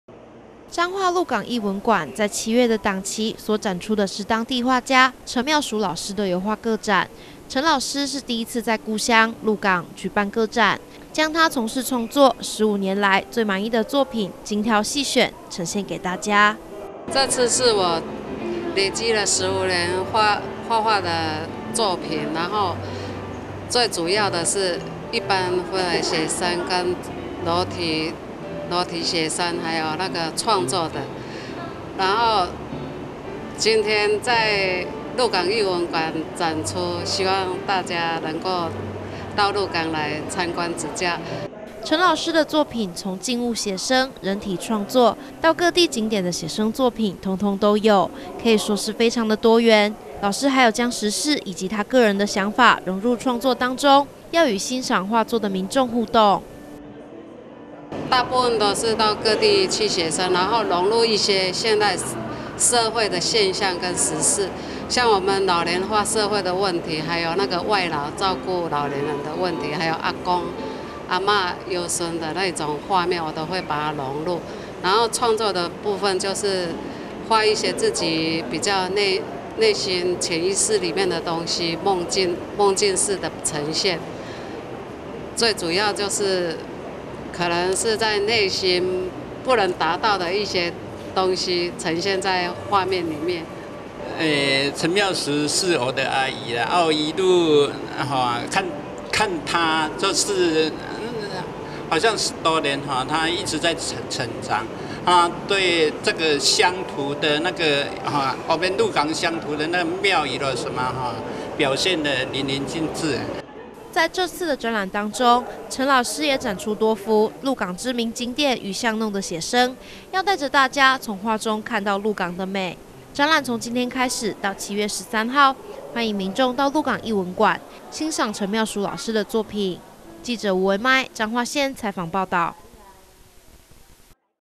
中台灣生活網-彰化鹿港藝文館陳妙叔油畫展
鹿港藝文館這個檔期，展出在地畫家陳妙叔老師的作品，這次是陳老師第一次在故鄉鹿港舉辦個展，展出的作品有從早期的靜物寫生、人體創作以及最近到各地寫生，內容多元豐富，由其有很多描繪鹿港知名景點的作品，由在地藝術家詮釋，代領大家一同感受鹿港之美，